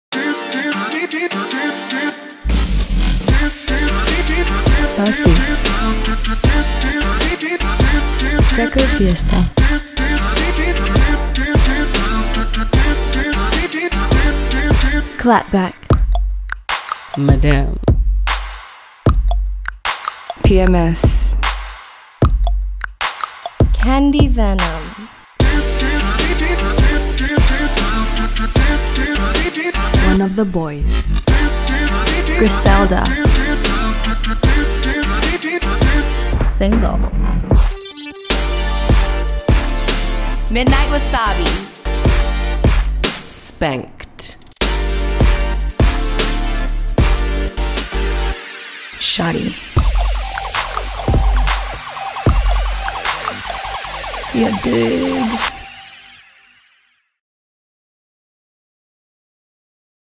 Fenty Beauty Mattemoiselle Looks
13 shades on 13 Malaysian girls